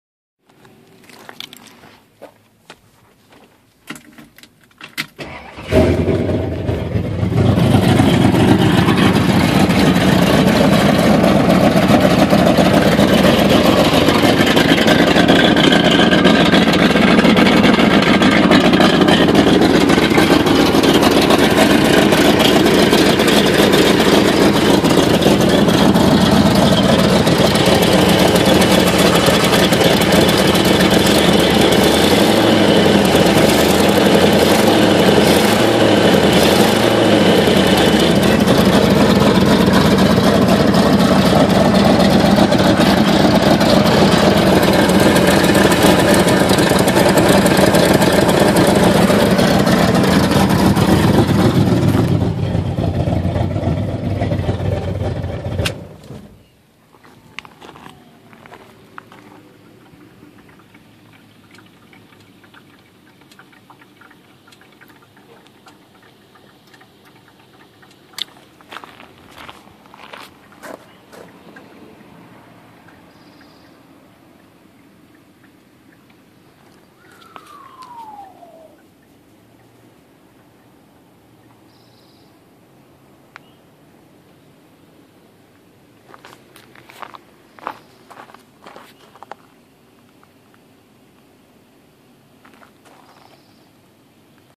Torino Turbo 1968 holiday weekend work
Welded on the front clip and started on the downpipe.